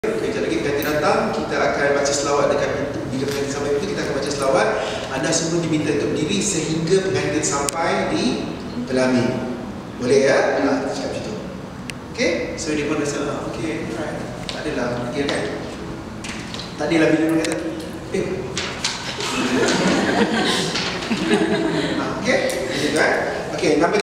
KURSUSPENGACARAANDZMS2016 - SEASON 4
Course Name: "I Wanna Be A Professional Emcee & TV Host: Intro to Hosting."

Objective: To encourage the youth to pursue their dream & generate extra income using their own hosting skill.